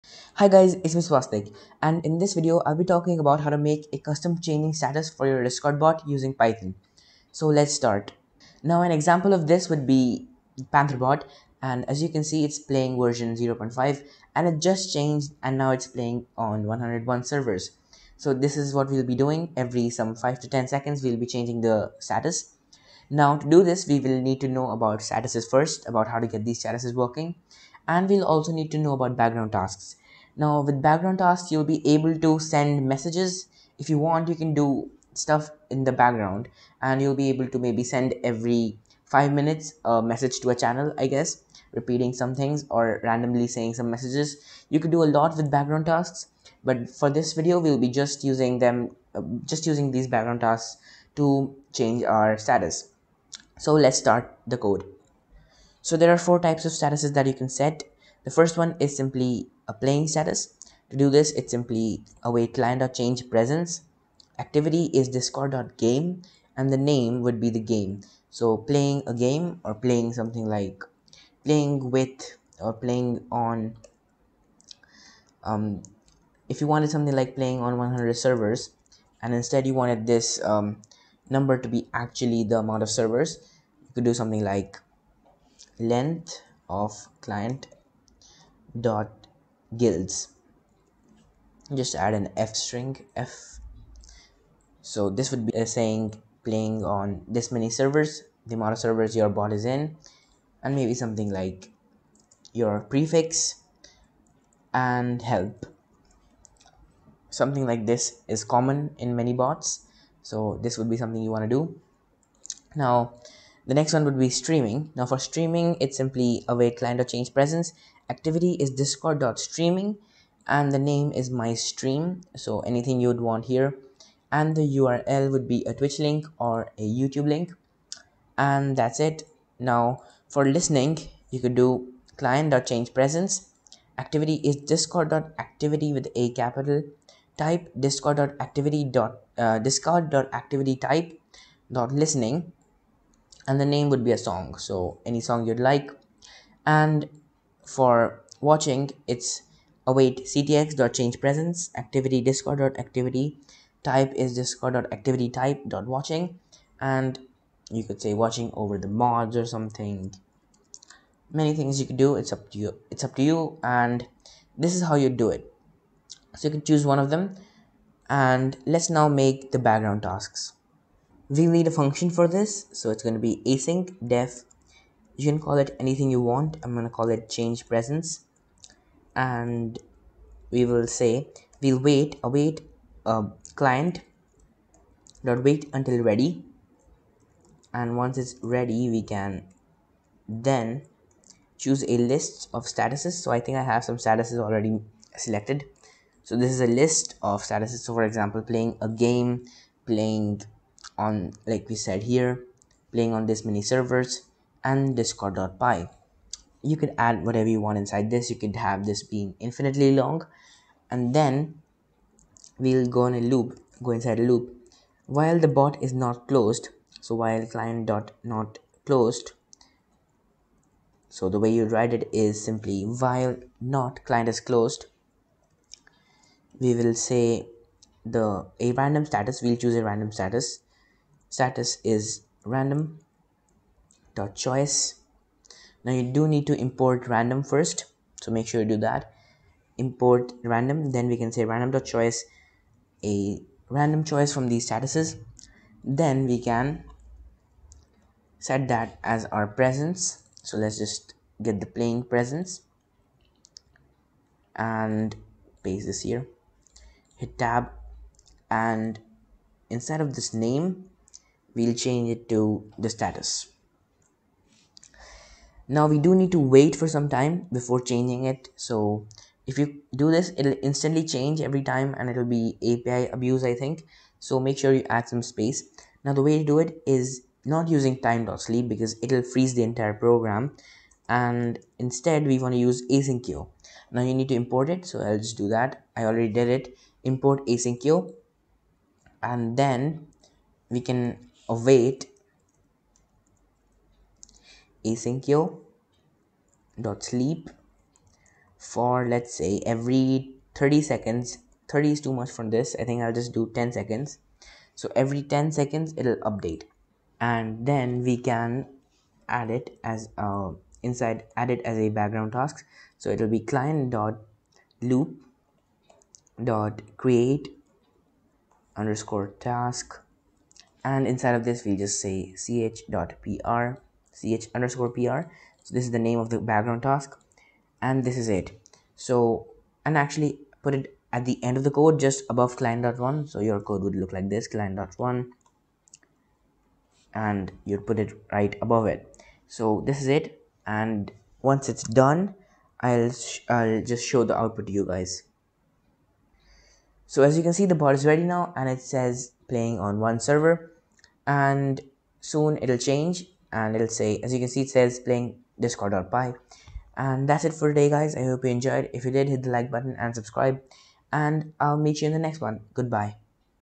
How to set a Custom Changing Status with discord.py | Background Tasks
In this video, we will learn how to set a Custom Changing Status for a Discord bot in python using discord.py (rewrite) in 2020.

If you have any suggestions for future videos, leave it in the comments below.

Queries solved in the video:
1) How to set a Custom Changing Status with discord,py
2) How to make a Background Tasks in Discord.py
3) How to set status with discord.py